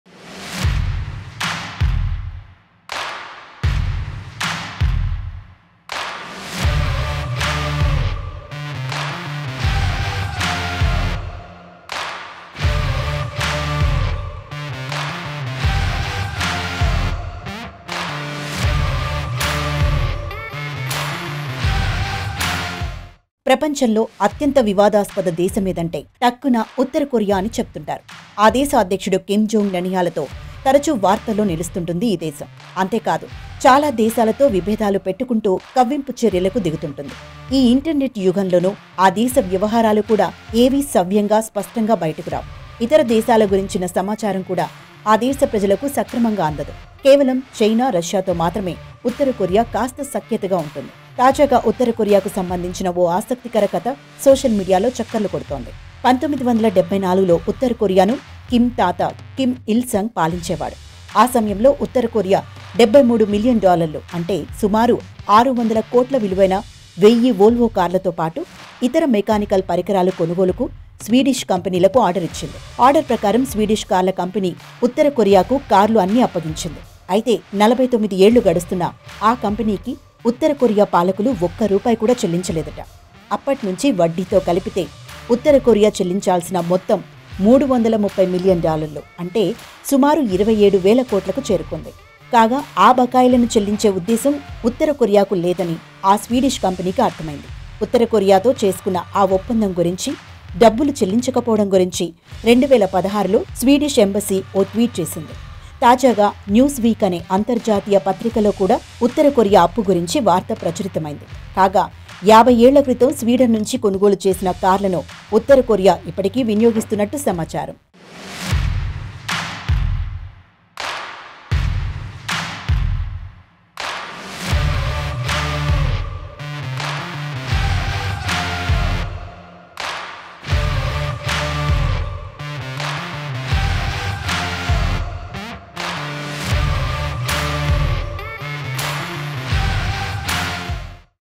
North Korea Ordered 1,000 Volvo cars from Sweden : 1000 కార్లు కొని డబ్బులు ఎగ్గొట్టిన నార్త్‌కొరియా
North Korea Cars : 1000 కార్లు కొని డబ్బులు ఎగ్గొట్టిన నార్త్‌కొరియా | ABN Digital Exclusives